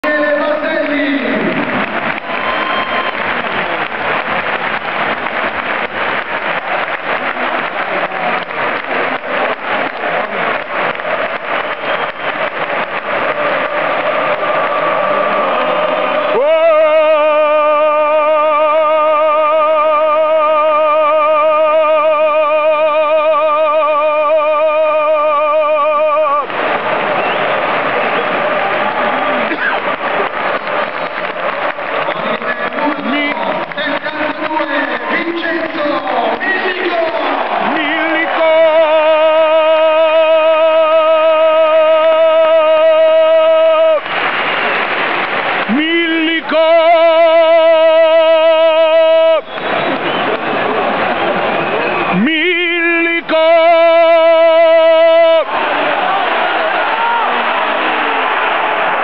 Torino Fc - Atalante 2-0 Esordio in Serie A di Vincenzo Millico
Esordio in Serie A di Vincenzo Millico